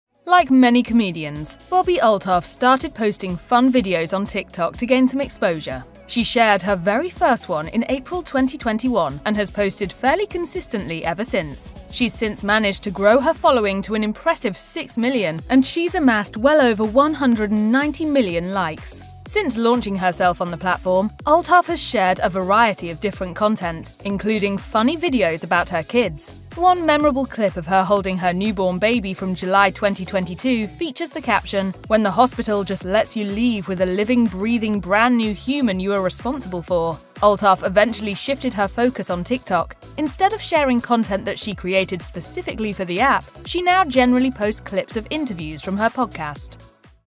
Bobbi Althoff's Rise To TikTok Stardom
If you keep up with pop culture in any way, chances are you've come across a video featuring a fully-dressed Drake and Bobbi Althoff, a comedian turned podcast host who has taken social media by storm thanks to her dry sense of humor and unique delivery of jokes.